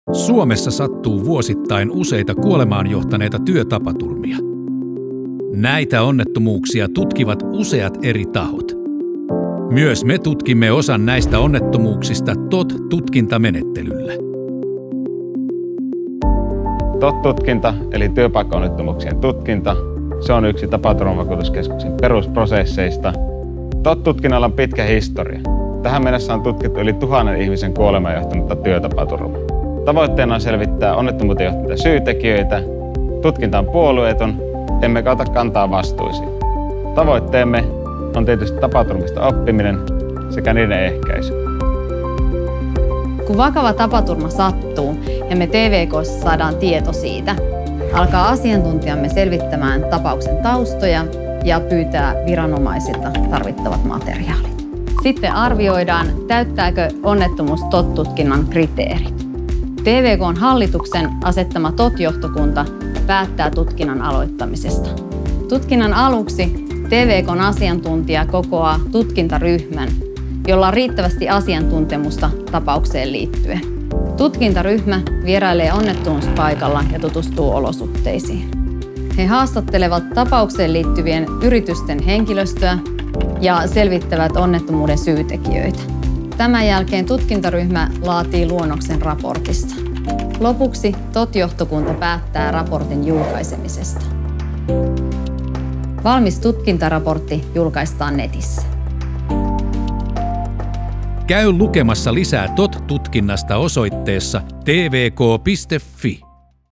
TOT-tutkinta tutuksi
Työturvallisuuden edistäminen ja työtapaturmista oppiminen. Mitä on TVK:n työpaikkaonnettomuuksien tutkinta eli TOT-tutkinta?